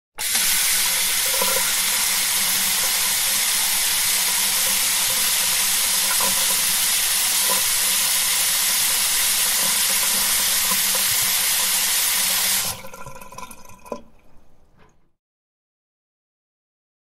Water Faucet Sound Effect | Free Sound Clips | Home and Office Sounds
Faucet Water Sound. Running Faucet Sound Effects. Great sound clip for video clips, games, commercials, apps. Free mp3 Download.